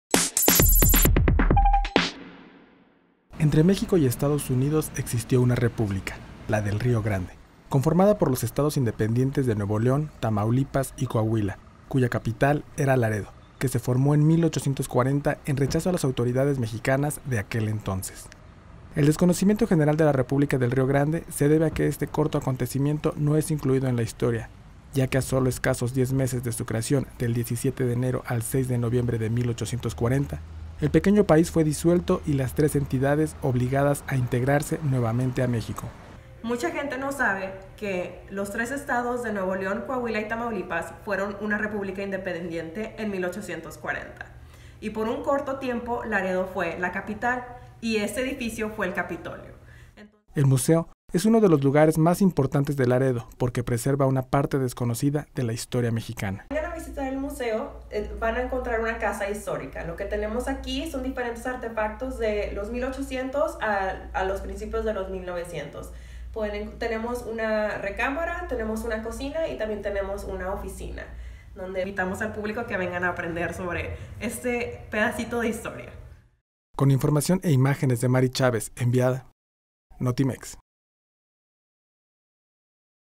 Entre México y Estados Unidos existió la República del Río Grande
El Museo de la Republica del Río Grande representa un pedazo de historia que no se conoce